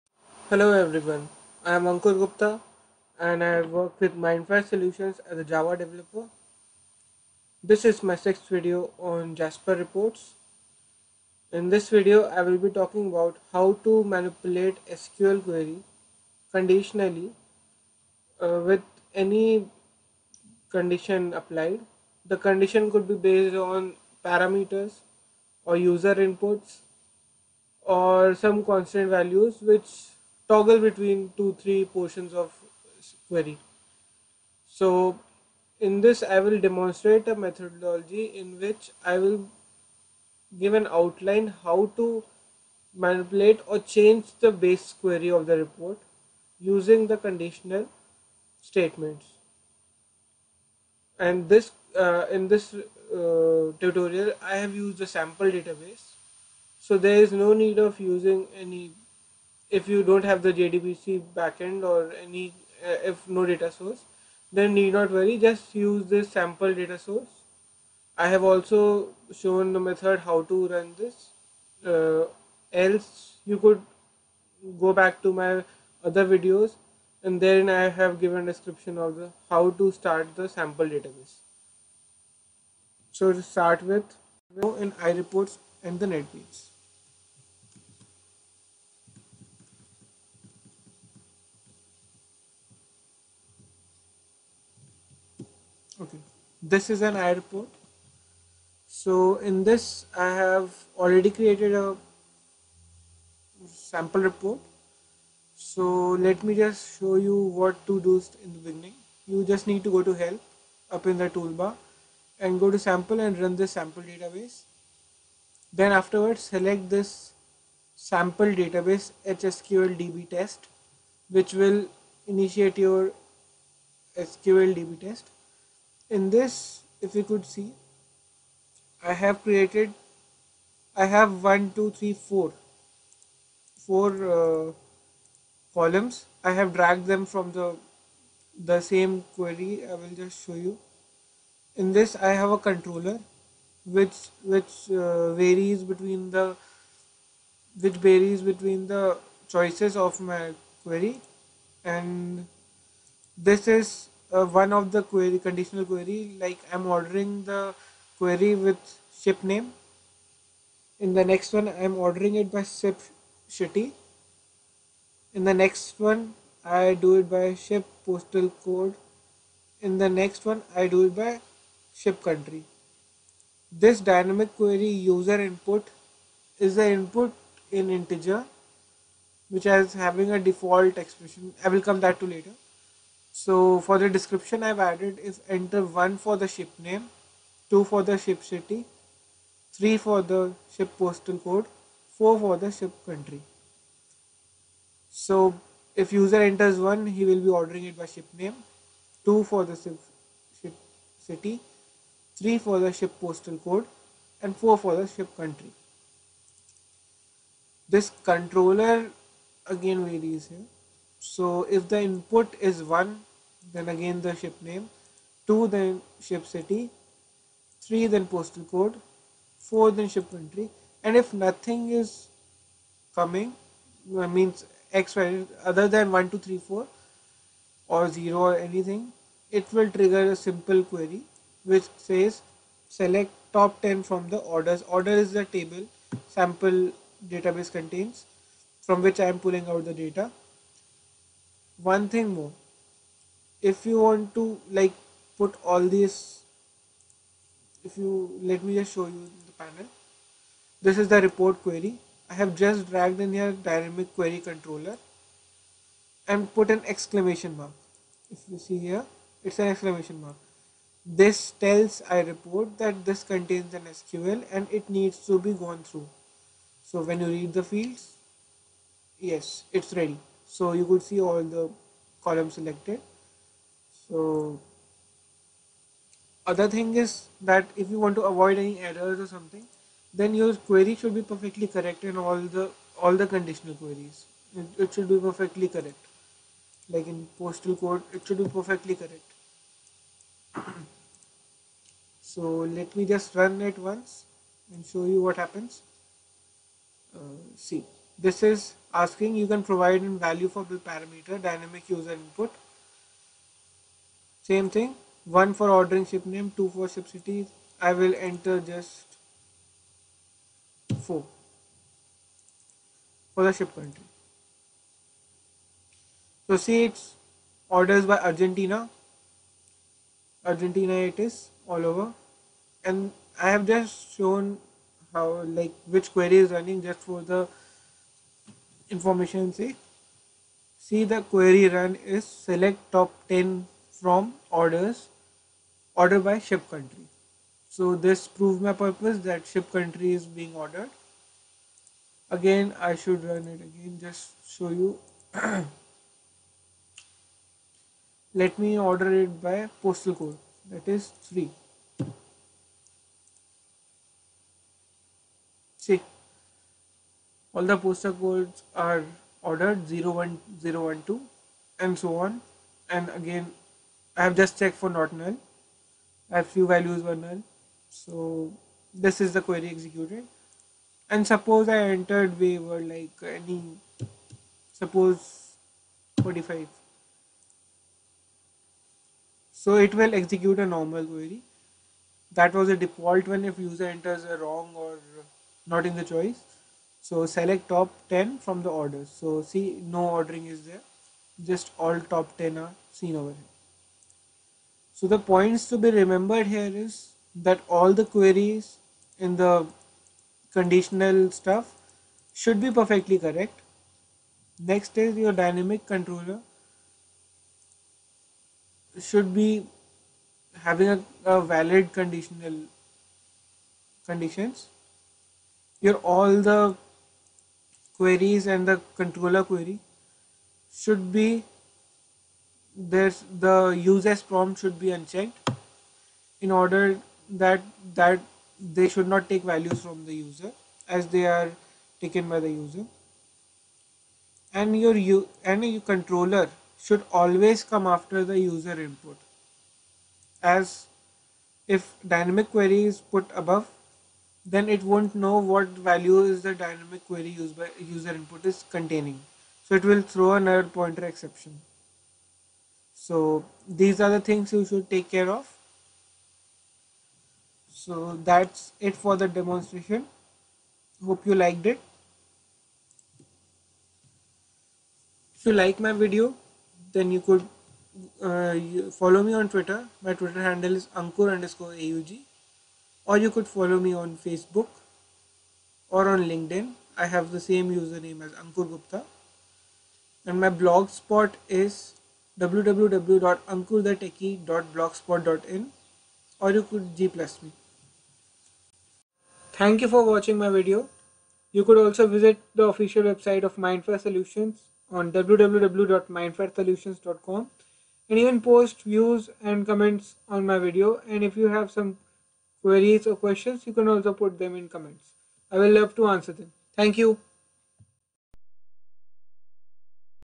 Tutorial to Implement Condition SQL in Jasper Reports Using iReport Tool
This video is explained a simple methodology to implement dynamic switching of SQL queries using parameters in iReports. More over the tutorial used the Sample Database of the iReports which comes preinstalled in the package so there is no need of setting up the backend in order to follow the tutorial, the requirement is just a simple installation of iReports.

Presenter: Ankur Gupta, Java Developer, Mindfire Solutions